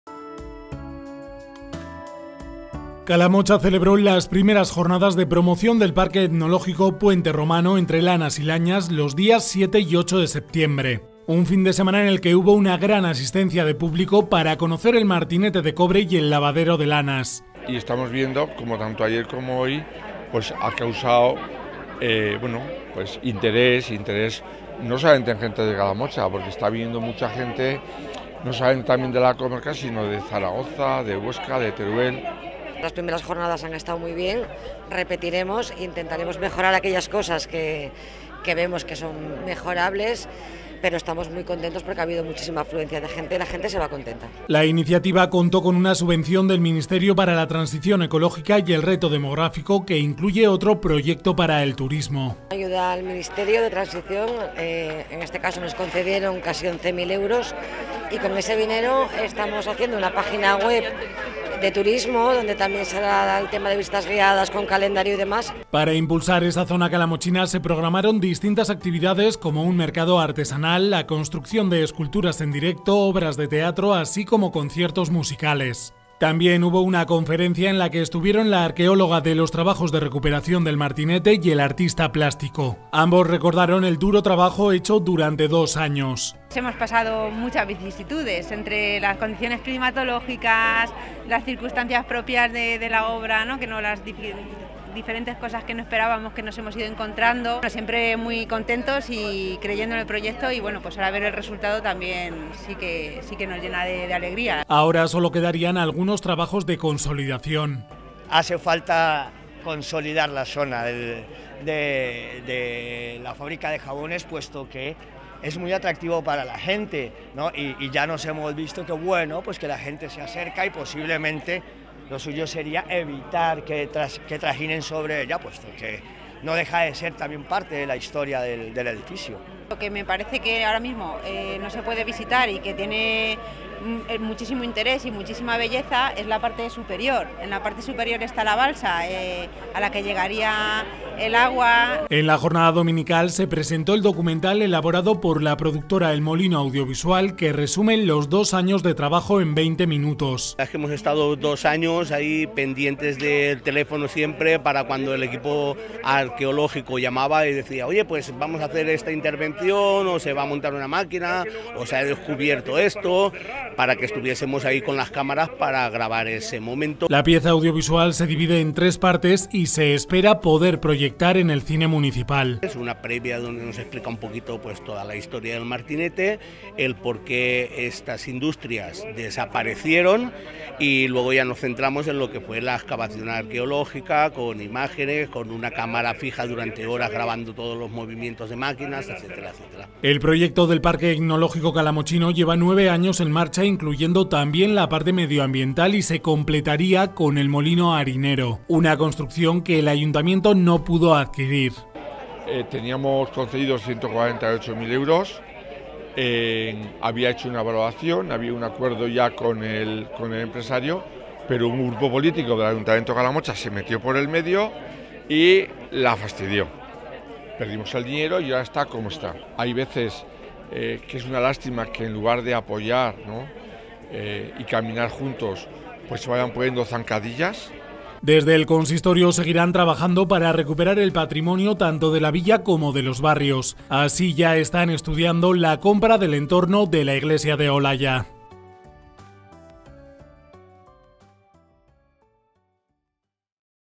Calamocha promociona su patrimonio industrial en "Entre Lanas y Lañas"
El Ayuntamiento de Calamocha a través de la Fundación San Roque desarrolló las primeras Jornadas de Promoción del Parque Etnológico Puente Romano de la villa. La iniciativa nominada "Entre Lanas y Lañas" contó con una amplia asistencia de vecinos.